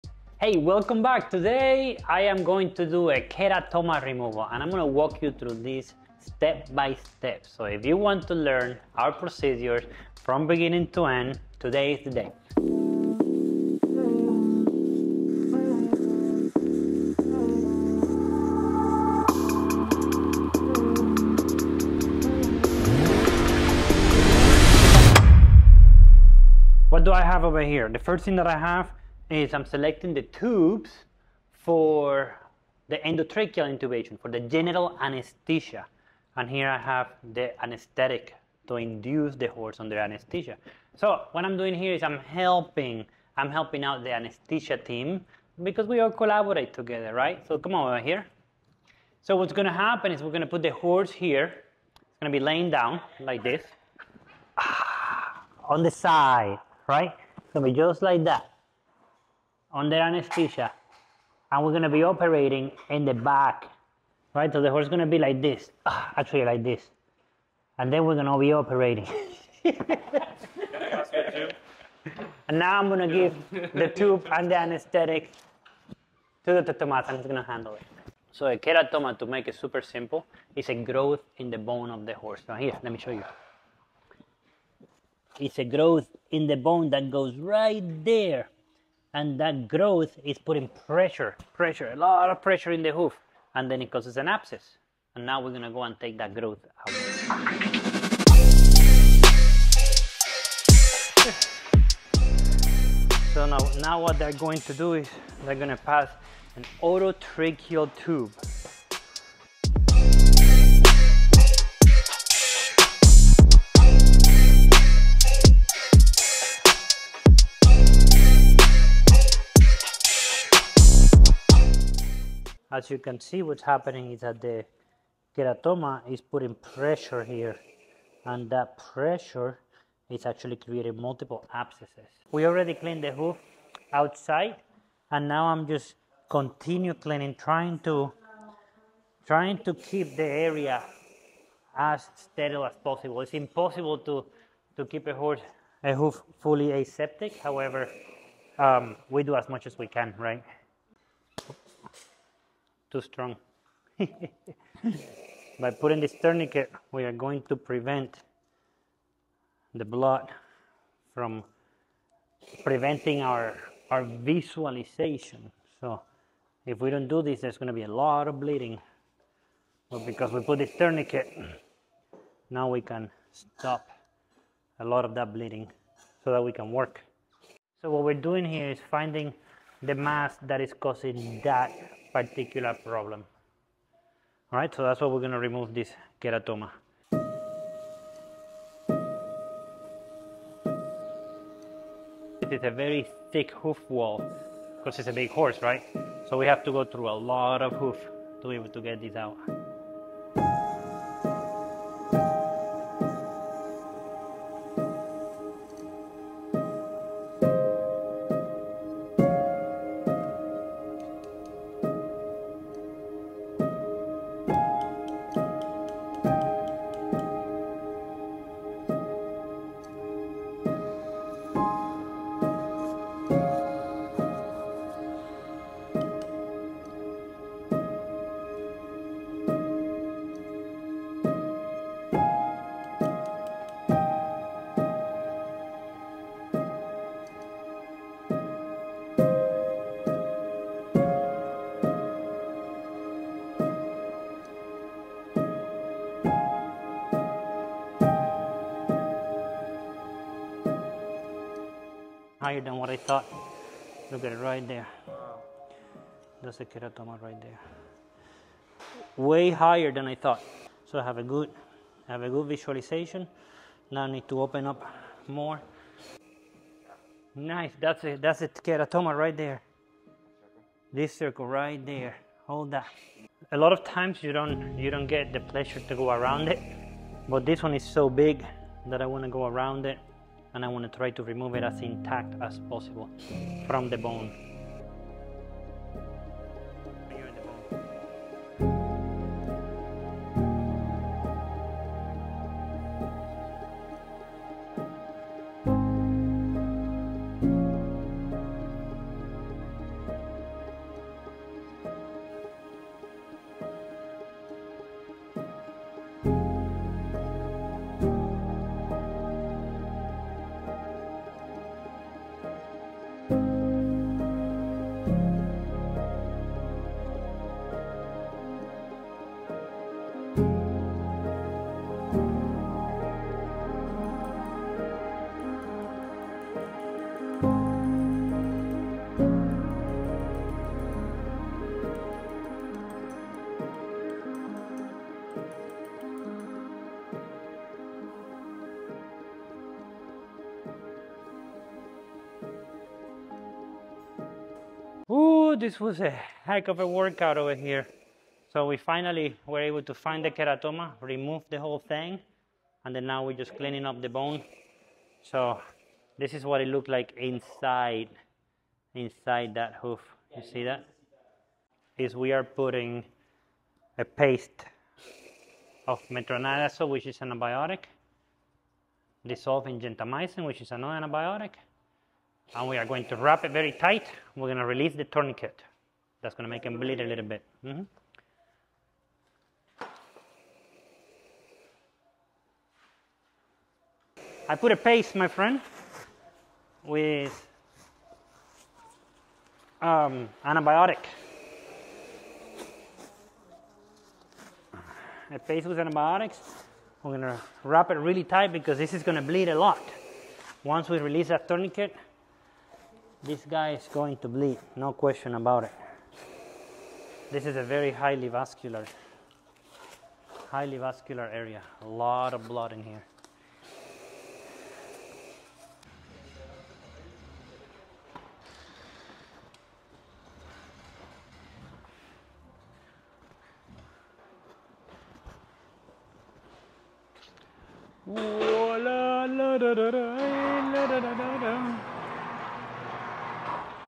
Keratoma Removal in Horse Hoof | Step-by-Step Equine Surgery Guide
In this video, we take you inside a real keratoma removal in a horse hoof, filmed at Equine Performance Innovative Center. If you've been searching for how keratoma removal in horses works, what causes it, or what equine surgery for hoof conditions looks like—this is the video to watch. Dr. Rullan walks through the entire process step-by-step, from the moment of diagnosis to hoof wall resection, surgical keratoma extraction, and wrapping for recovery.

Keratomas are masses that form inside the hoof and lead to chronic abscesses, lameness, and pain. This case involved a large keratoma deeply embedded in the hoof wall, requiring a full hoof wall resection and removal under general anesthesia. You'll see how tourniquets are used to control bleeding, how the hoof is stabilized, and how post-op antibiotics are administered to protect the surgical site.

Whether you're a vet student, equine podiatrist, farrier, or horse owner looking to understand keratoma removal in horse hooves, this is the most detailed surgical walkthrough online.